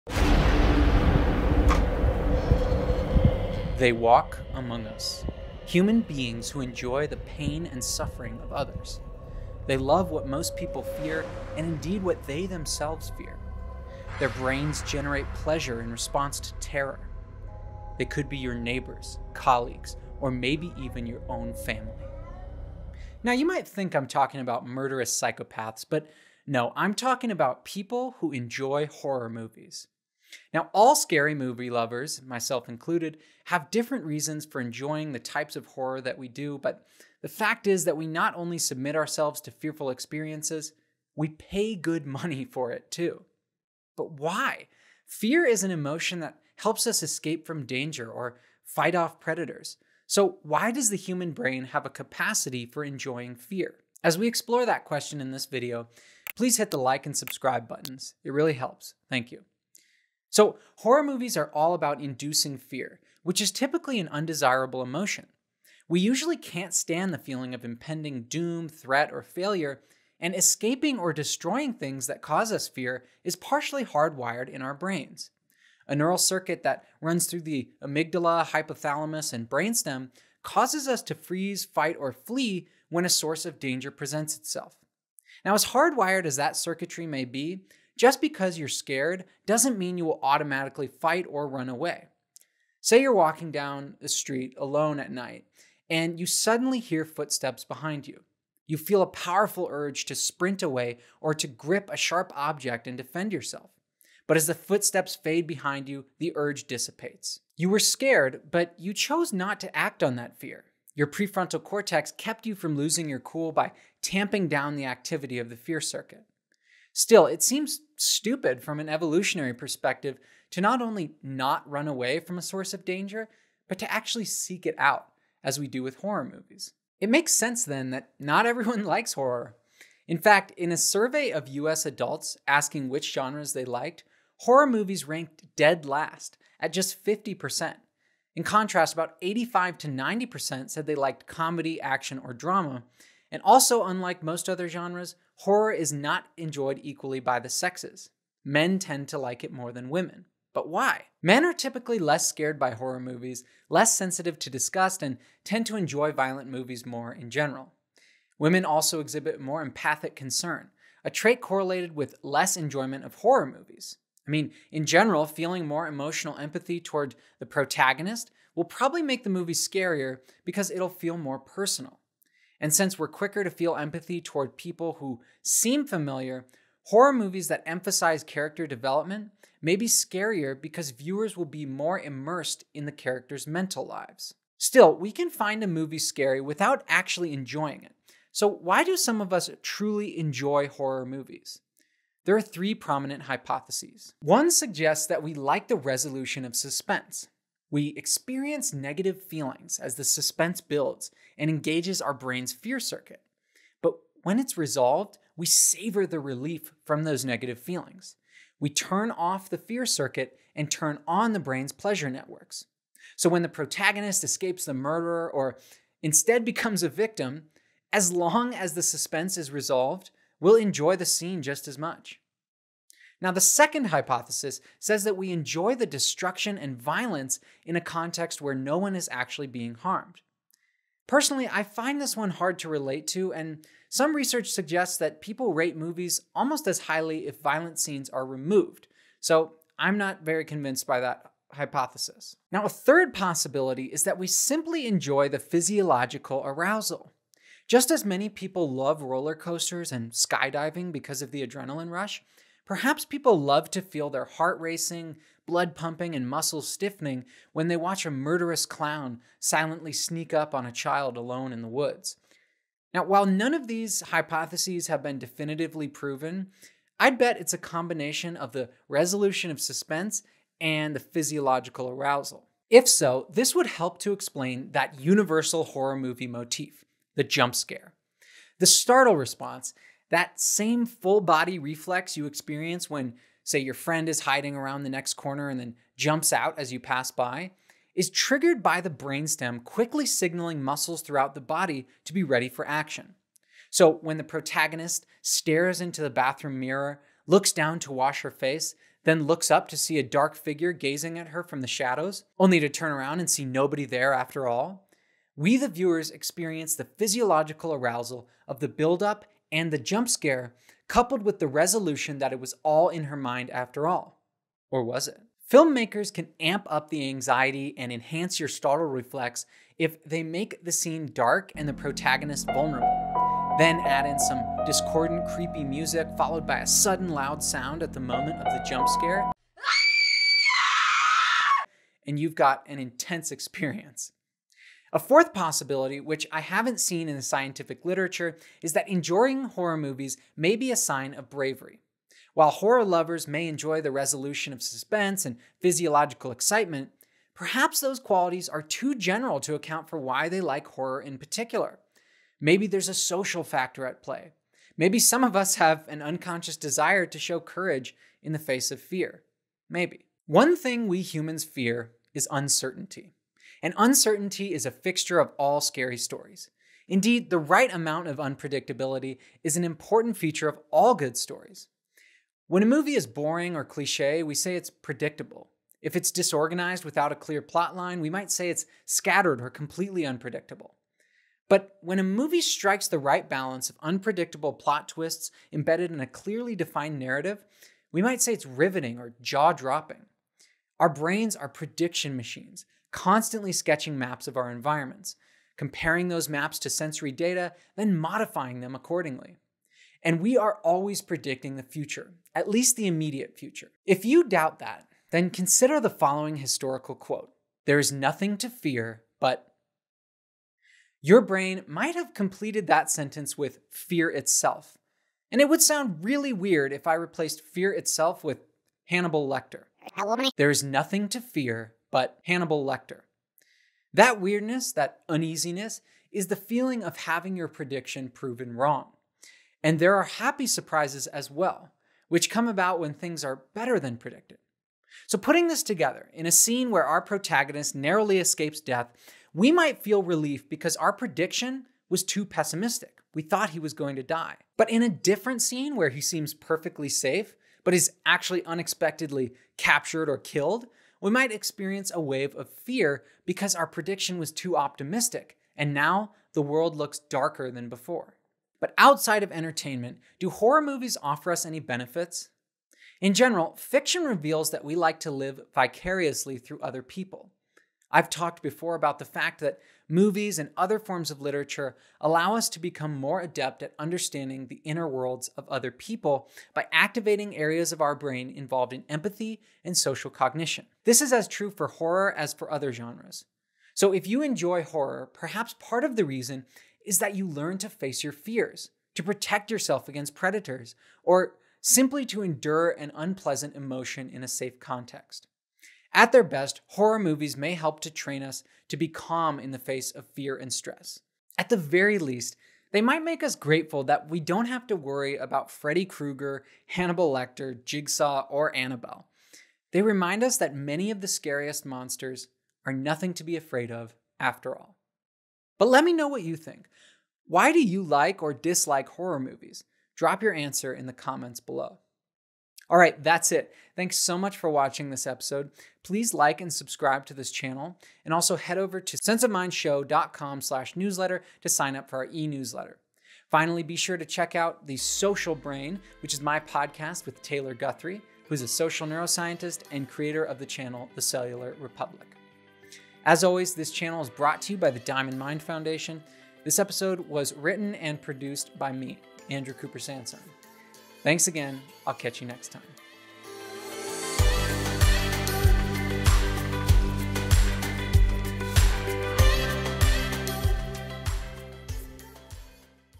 Horror Movies MANIPULATE YOUR BRAIN (the neuroscience of scary movies)
Horror movies can subtly manipulate the human brain, causing us to feel fear when there is not actually any danger. Scary movies do this by stimulating our brain’s fear circuit. But that’s pretty obvious. The more important question is: Why do people voluntarily submit themselves to such a scary experience? Why do some people enjoy horror movies while others can’t stand it? How do horror movies take advantage of our brain’s design to make us simultaneously feel terrified while knowing that nothing bad is actually happening? This video addresses all of those questions. At the end, I describe some unintuitive benefits of watching scary movies.

Chapters:
00:00 The neuroscience and psychology of horror movies
07:38 Benefits of horror movies

Sources:
- Martin GN (2019) (Why) Do You Like Scary Movies? A Review of the Empirical Research on Psychological Responses to Horror Films. Front. Psychol. 10:2298. doi: 10.3389/fpsyg.2019.02298

- Nummenmaa, L. (2021). Psychology and neurobiology of horror movies. PsyArxiv. Preprint.

- Panksepp, J., & Biven, L. (2012). The archaeology of mind: neuroevolutionary origins of human emotions (Norton series on interpersonal neurobiology). WW Norton & Company.